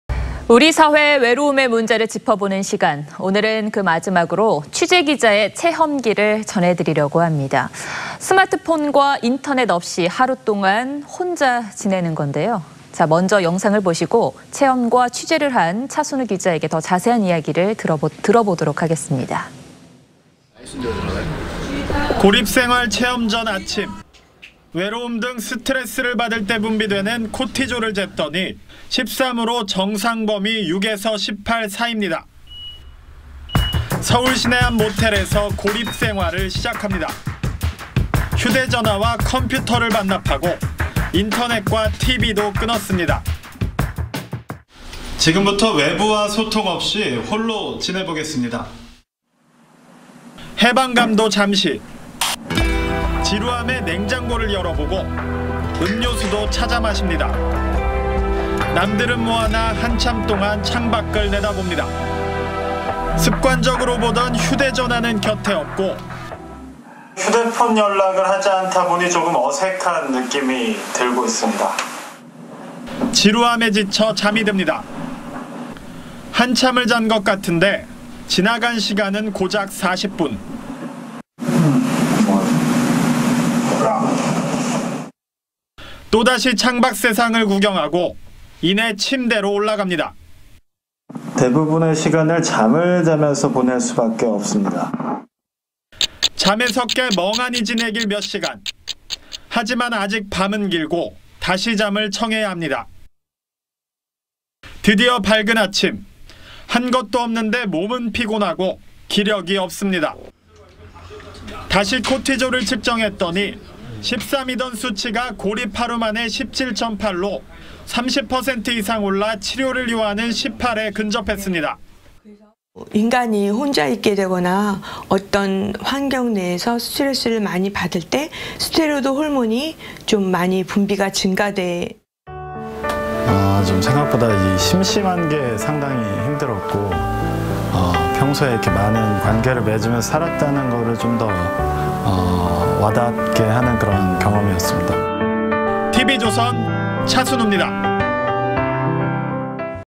'단 하루 고독' 체험했더니 외로움 스트레스 급상승
우리 사회 외로움의 문제를 짚어보는 시간, 오늘은 마지막으로, 취재기자의 체험기를 전해드리려고 합니다. 스마트폰과 인터넷 없이, 하루 동안 혼자 지내는건데요, 먼저, 영상을 보시고, 체험과 취재를 한 차순우 기자에게 더 자세한 이야기를 들어보도록 하겠습니다.